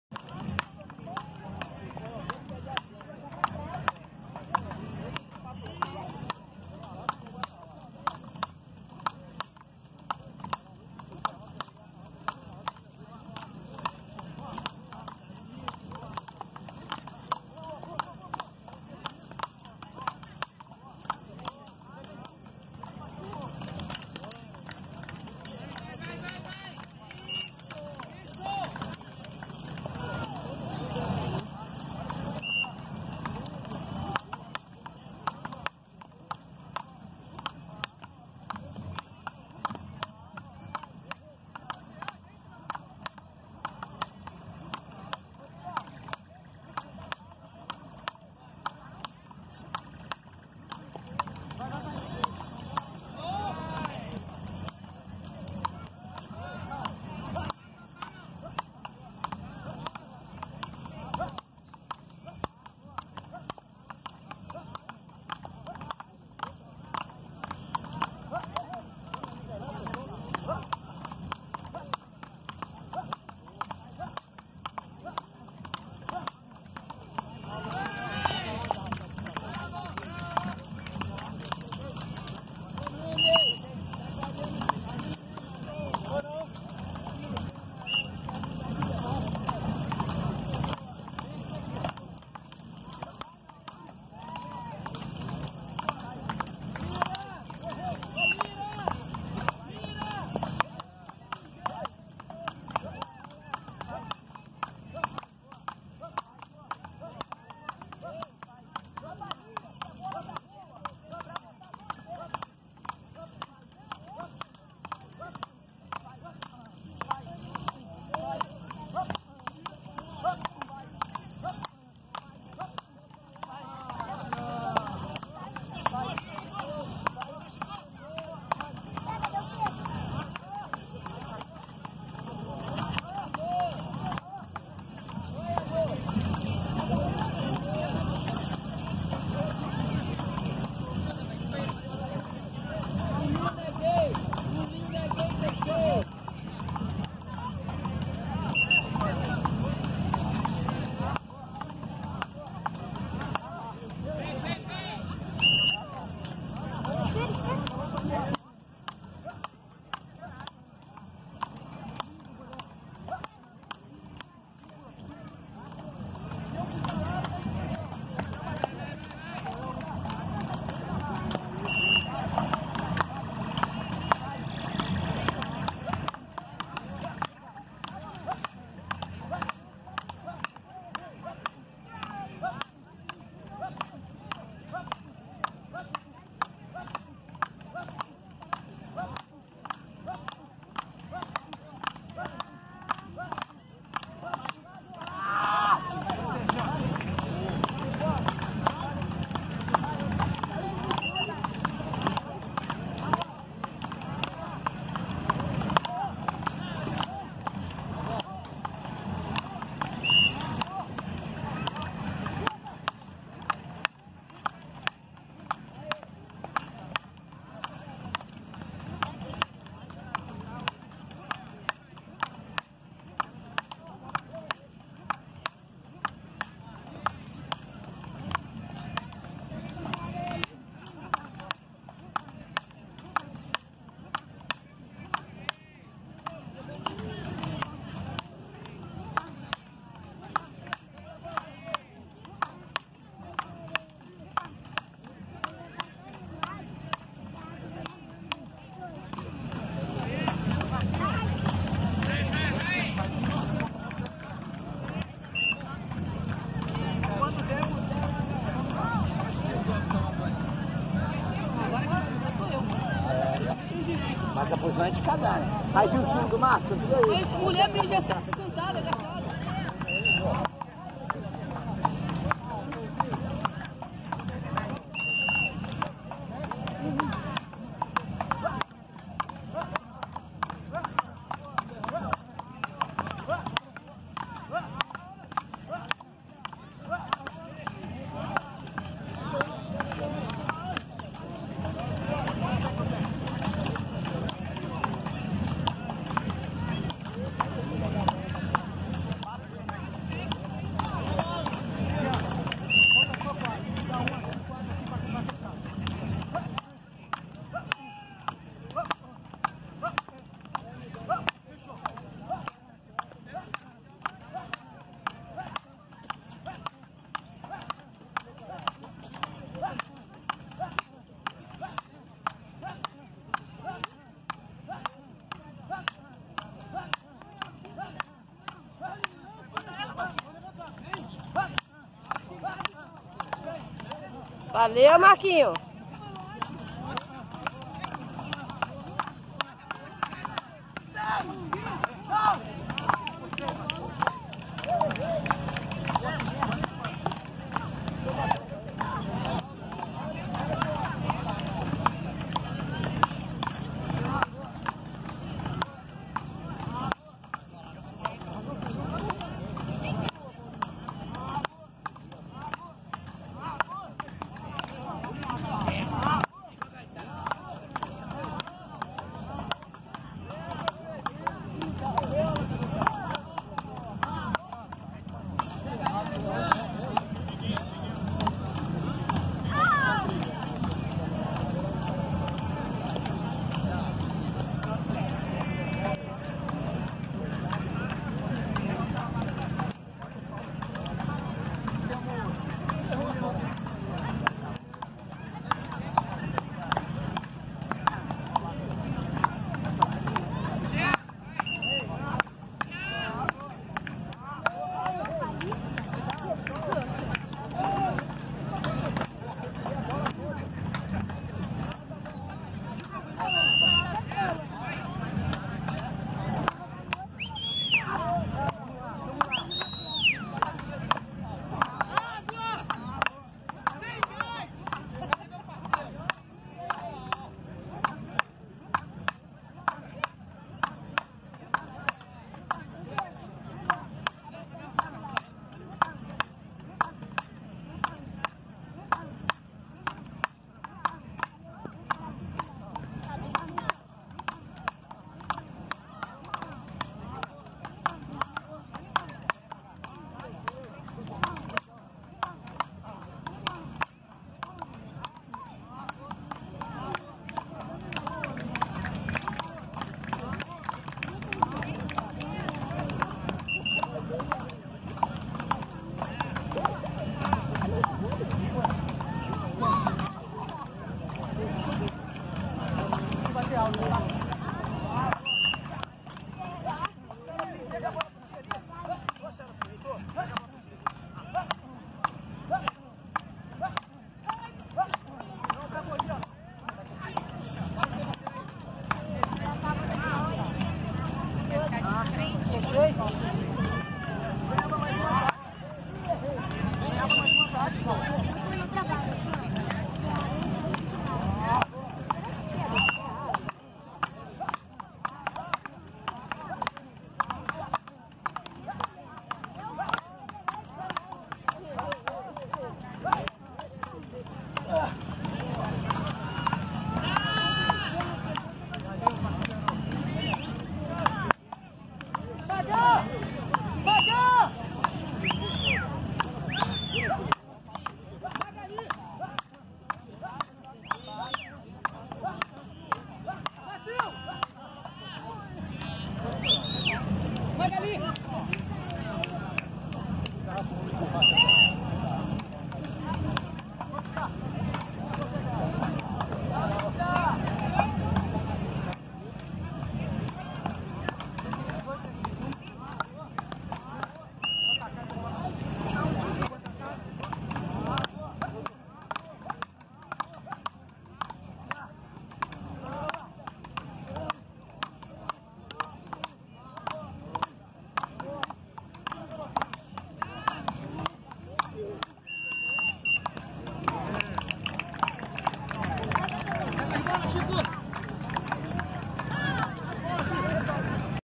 FRESCOBOL JEAN E MARCOS SANTANA - ABRAF ETAPA COPACABANA - DEZ 18 - FINAL
Classificação final 4 lugar categoria masculina